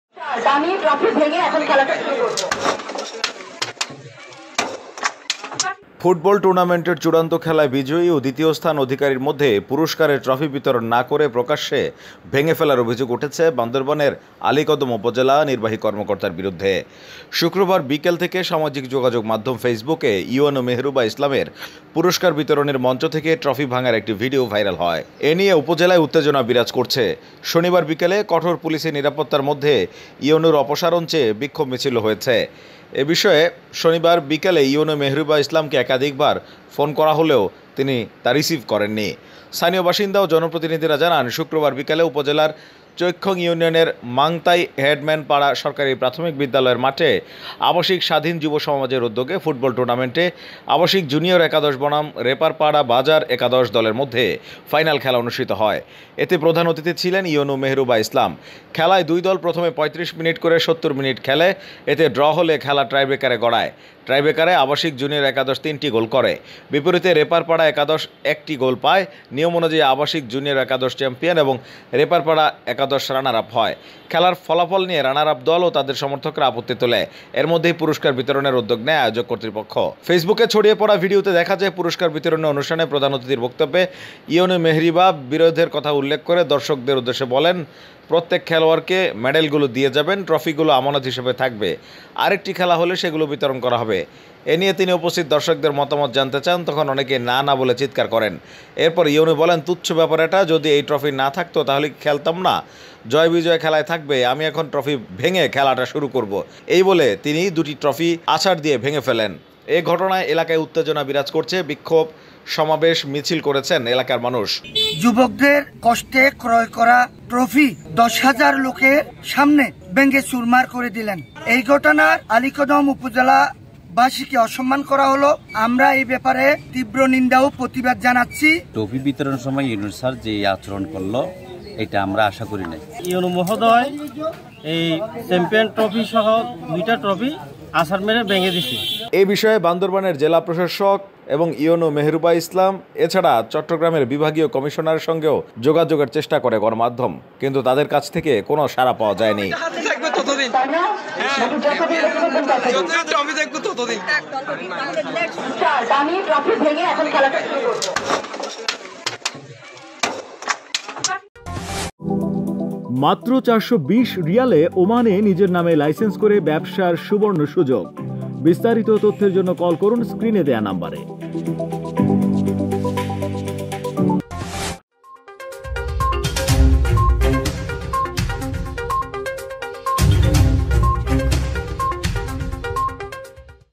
বক্তব্যের সময় মেজাজ হারিয়ে আছাড় দিয়ে ট্রফি ভাঙলেন ইউএনও | UNO Trophy | Probash Time
ফাইনাল ফুটবল খেলায় প্রধান অতিথি হিসেবে উপস্থিত ছিলেন আলীকদম উপজেলা নির্বাহি অফিসার মেহরুবা ইসলাম। উক্ত খেলায় সমাপনী বক্তব্যের সময় হঠাৎ জনগণের উপর ক্ষিপ্ত হয়ে কোমলমতি ছেলেদের কষ্টে কেনা ফাইনাল খেলার ট্রফি ভেঙ্গে ফেলেন।

- Team Probash